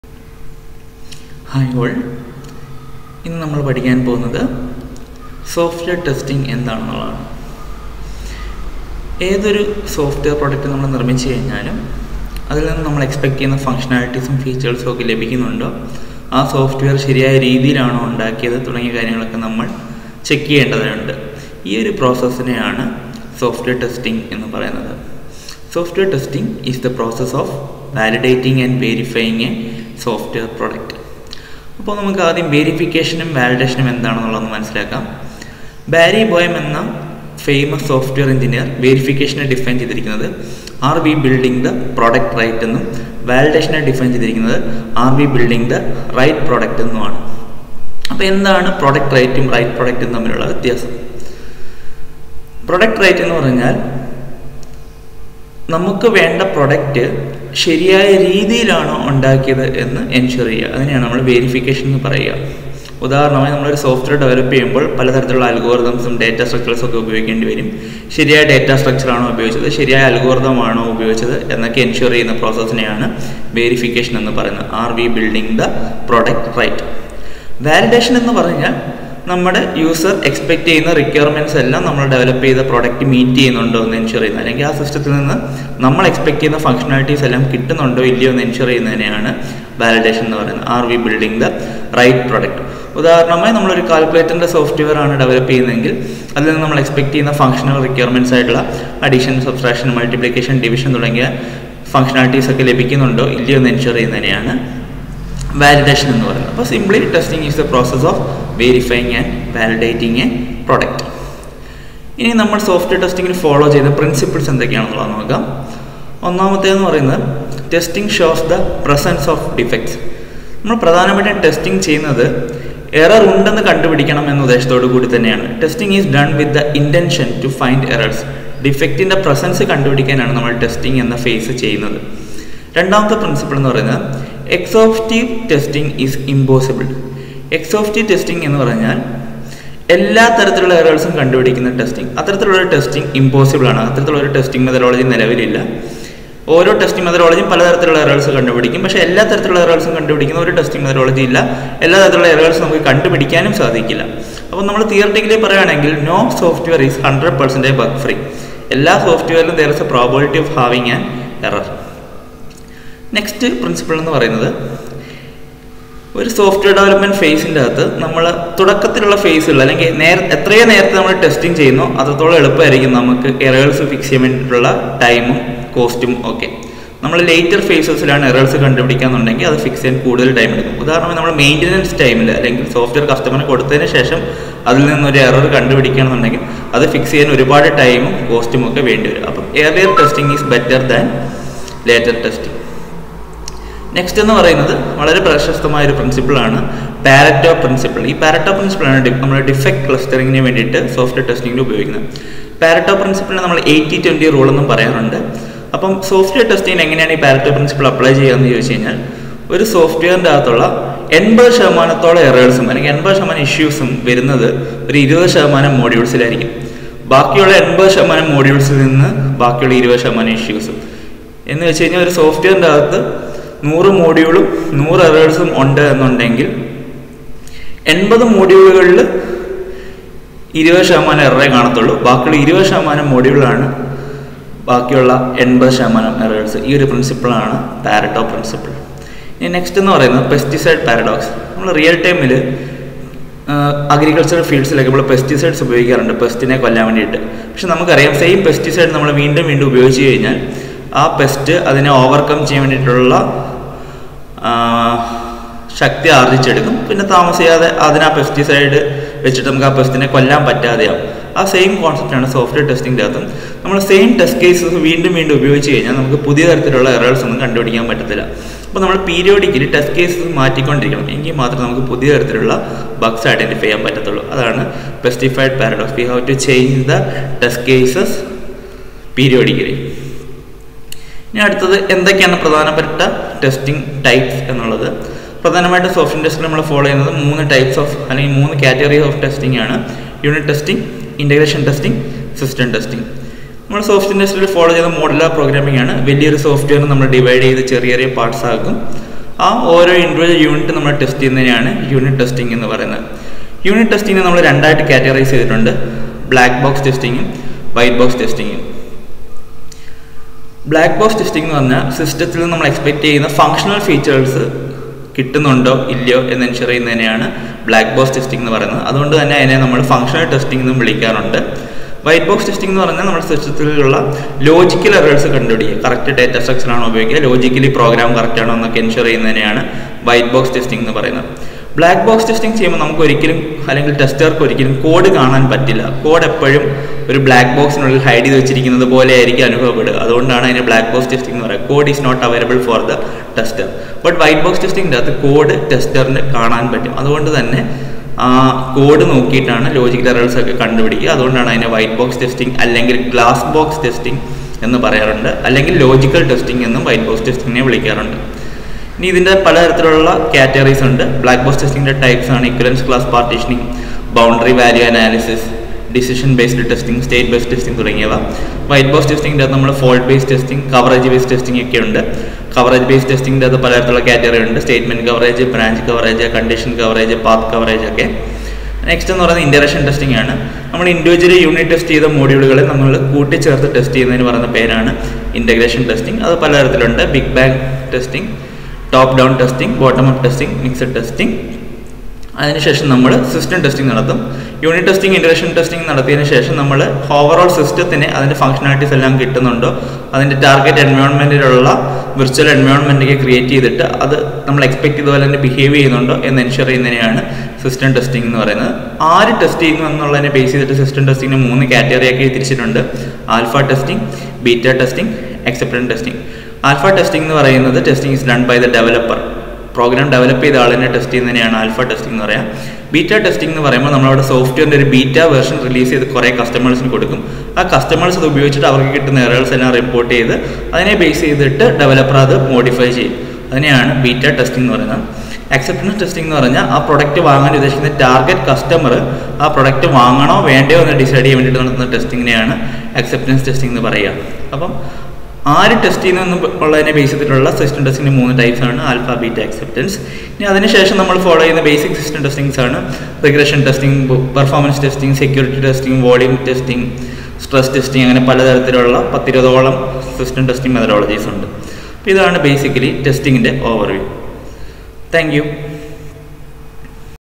Software Engineering & Project Management - Software Testing Part 1
This video provides the overview of Software Testing and the Principles of Testing